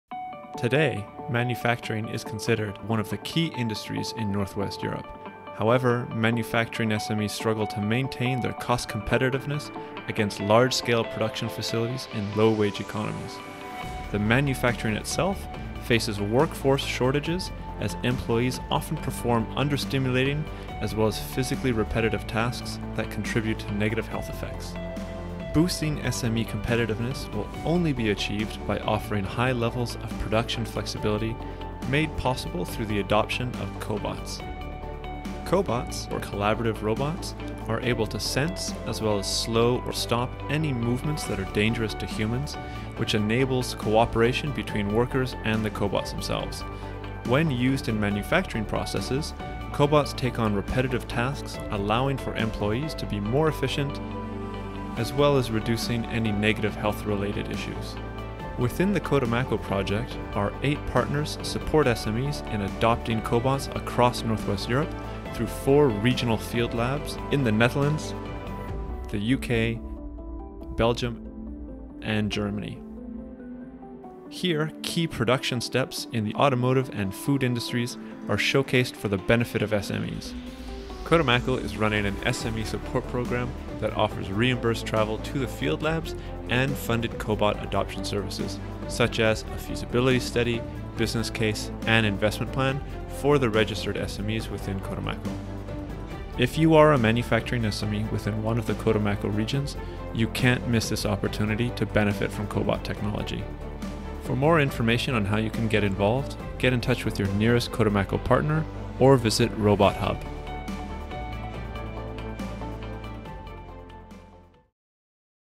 How collaborative robots increase the competitiveness of SMEs.
Human-robot collaboration helps SMEs face the challenges linked to high cost competition. Production flexibility gives them a clear advantage. Within the COTEMACO project, 8 partners in North-West Europe help SMEs implement cobot-technology. Discover here how they can help you.